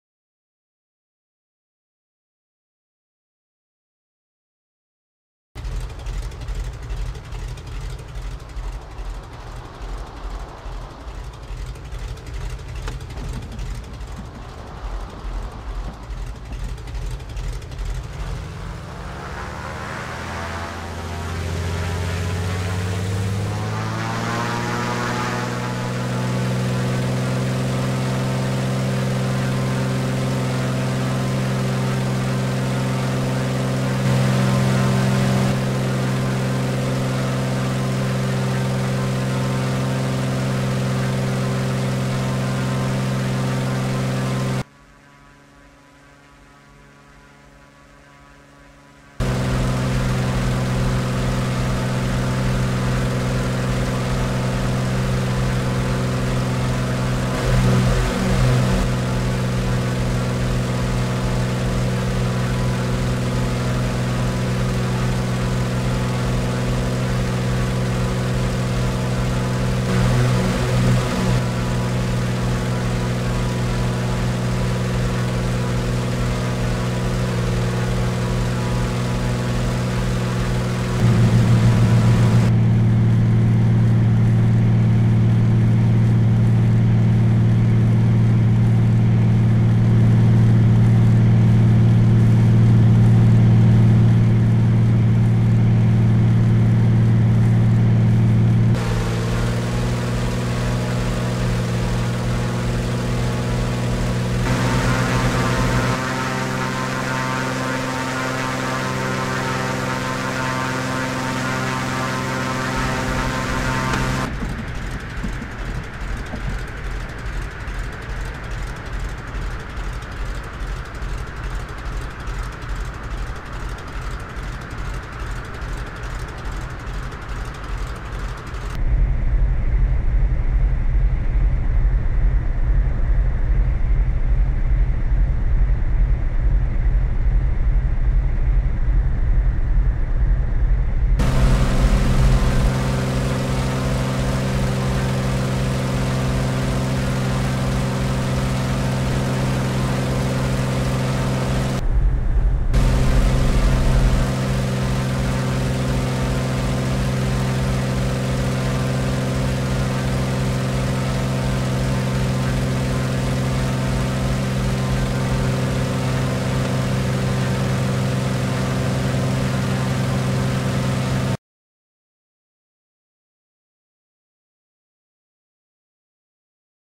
HD WATER EFFECTS Take off/Landing
This is a take off from Juneau (JNU) and a landing next to the runway of an airport nearby. I used a de Havilland Beaver DHC2 Kenmore Air. I am using water effects from REX 2.0.